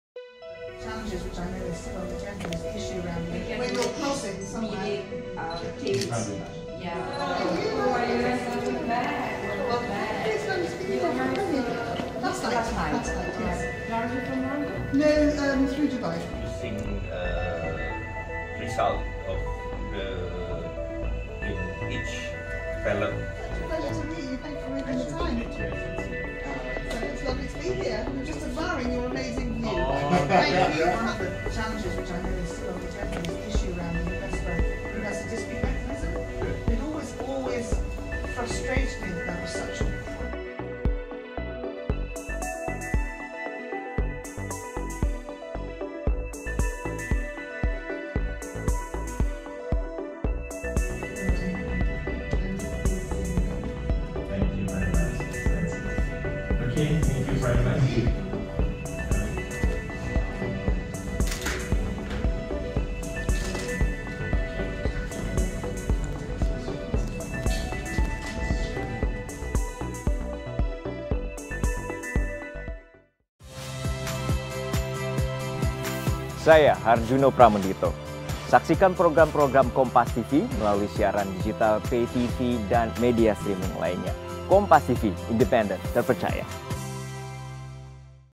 Momen Menko Airlangga hingga Menlu Retno Bertemu Menteri Inggris untuk Indo Pasifik
JAKARTA, KOMPAS.TV - Menteri Inggris untuk Indo Pasifik Anne-Marie Trevelyan bertemu dengan Menko Perekonomian Airlangga Hartarto, Menteri Luar Negeri (Menlu) Retno Marsudi dan Wamenkominfo Nezar Patria saat kunjungan kerja ke Indonesia, Kamis (29/2/2024).

Trevelyan membahas kemitraan antara Inggris dan Indonesia di berbagai sektor saat bertemu Airlangga.

Lalu, pertemuannya dengan Retno Marsudi membahas terkait isu-isu penting kebijakan luar negeri antara dua negara.

Sementara itu, Trevelyan membahas literasi digital, transformasi digital, kecerdasan buatan hingga penanggulangan disinformasi saat bertemu dengan Wakil Menteri Kominfo (Wamenkominfo) Nezar Patria.

Baca Juga Airlangga Bicara Soal Peran Jokowi di Pemerintahan Baru di